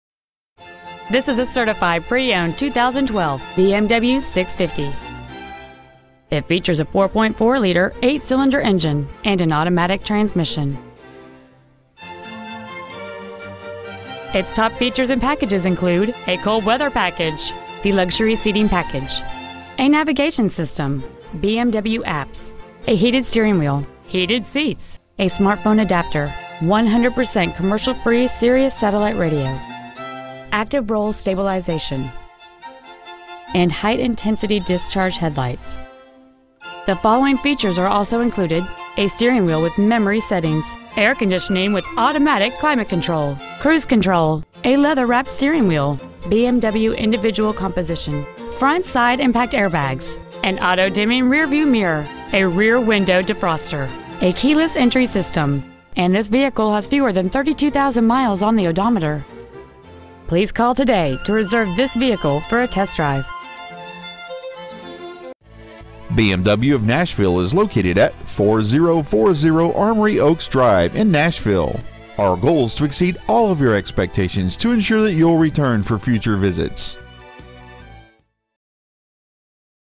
2012 BMW 650i Convertible Certified Nashville TN 37204
We've been honored to serve the Nashville TN area , we promise that your experience at our dealership will exceed your expectations ! Year : 2012 Make : BMW Model : 650i Convertible Engine : 4.4L V-8 cyl Trans . : Automatic Exterior : Black Sapphire Miles : 31,937 Interior : Ivory White BMW of Nashville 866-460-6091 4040 Armory Oaks Drive Nashville , TN 37204 Pre-Owned 2012 BMW 650i Convertible Nashville TN